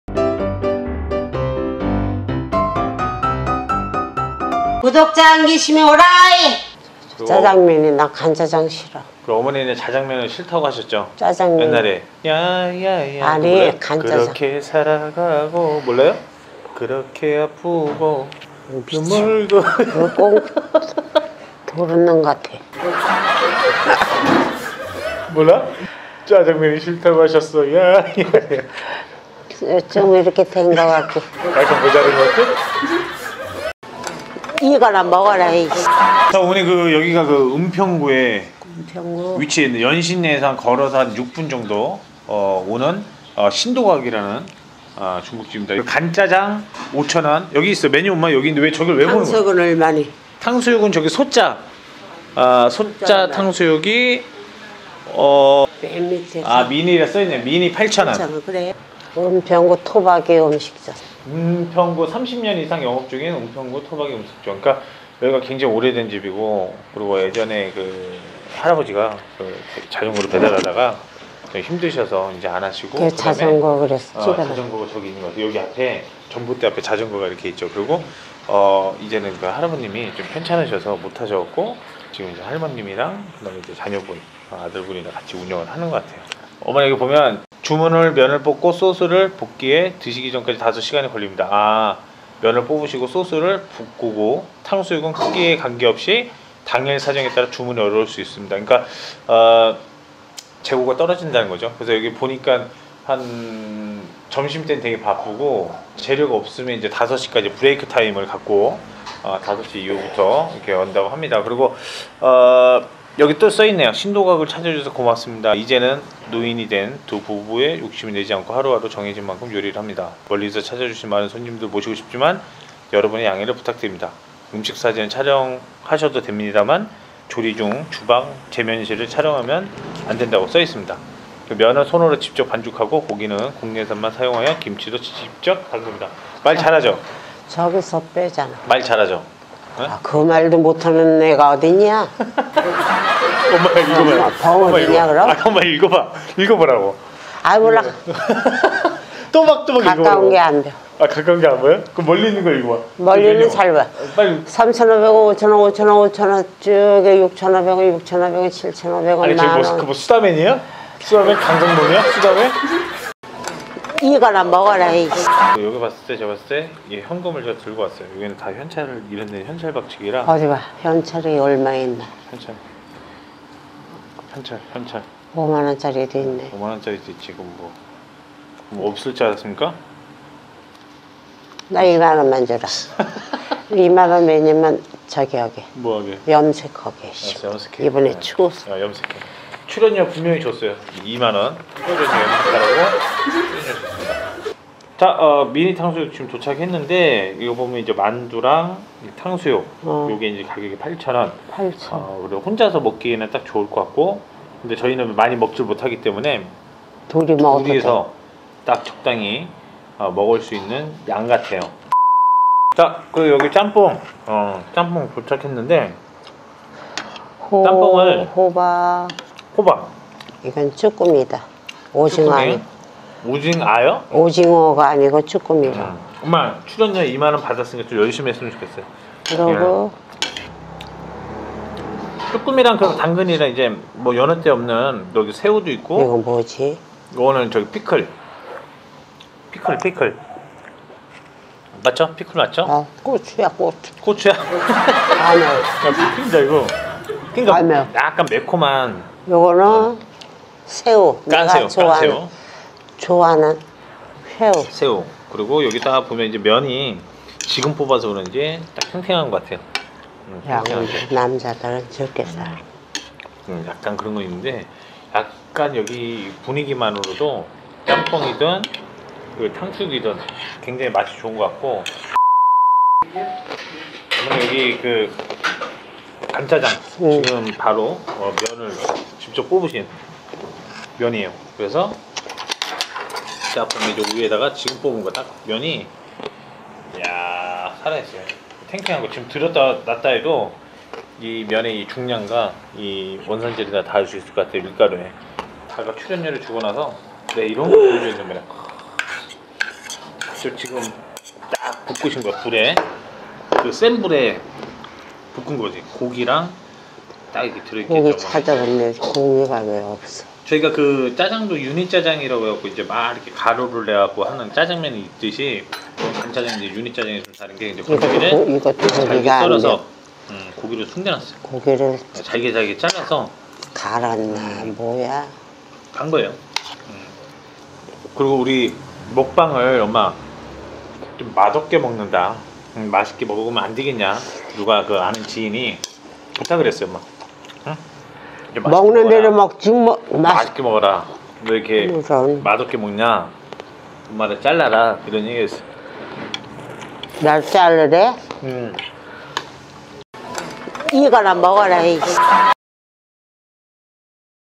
43년 경력의 노부부 간짜장 맛집 (연신내 신도각)은평구맛집
70대 어머니와 떠나는 먹방여행~!!
서울시 은평구 대조동에 위치한 43년된 노포 중국집
한메뉴만 팔린다는 중국집에 가보았습니다.